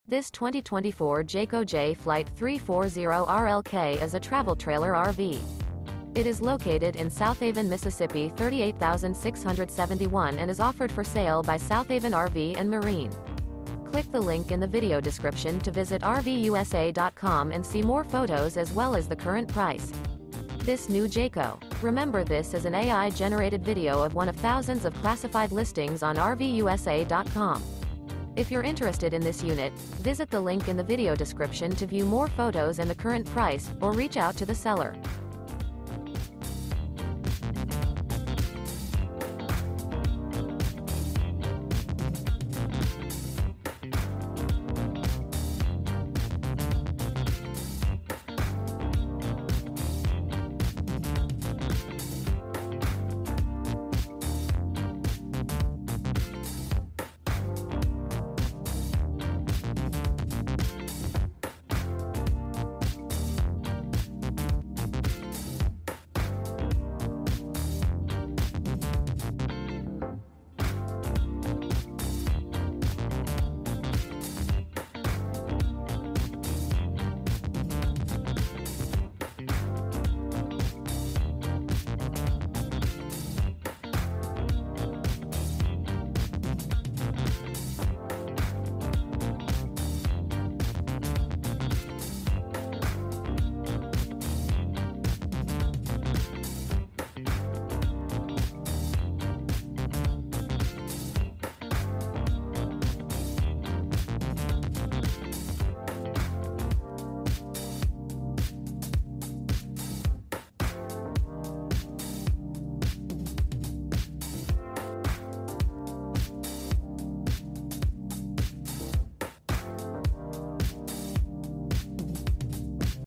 Unbelievable 2024 Jayco Jay Flight Travel Trailer RV For Sale in Southaven, MS | RVUSA.com
This 2024 Jayco Jay Flight 340RLK is a Travel Trailer RV. It is located in Southaven, MS 38671 and is offered for sale by Southaven RV & Marine. This New Jayco.